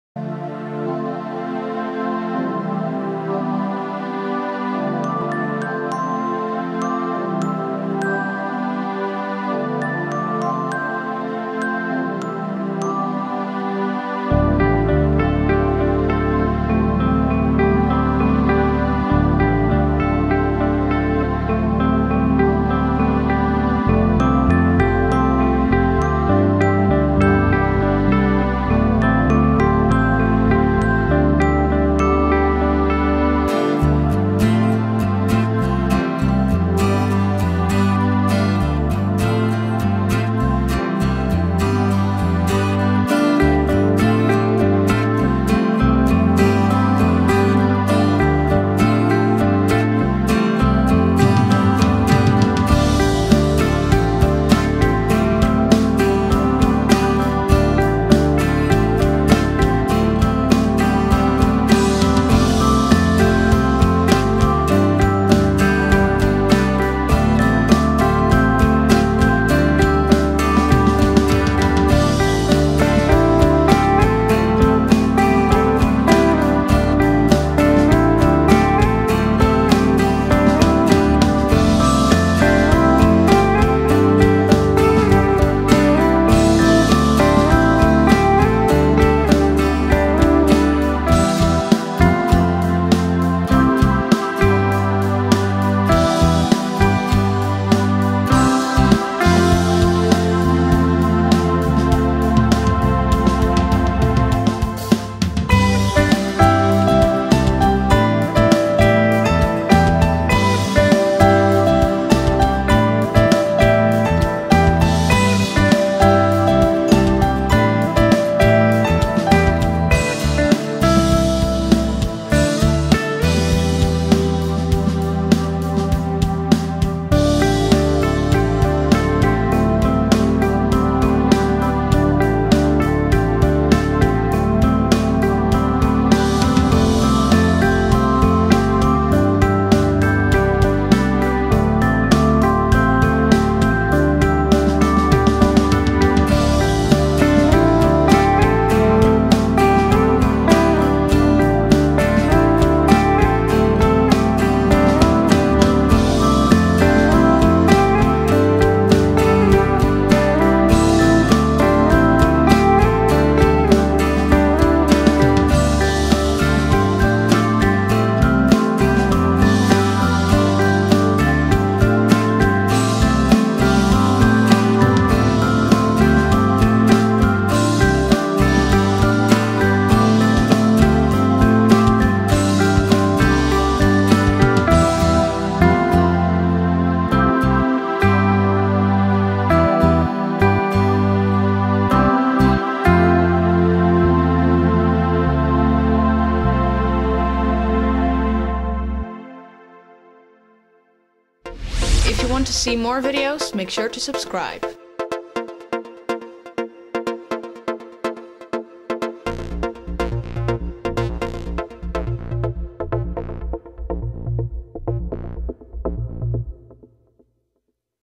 Bedrijfsfilm trekkerweb
Onze Open Dagen 2014. Een mooi overzicht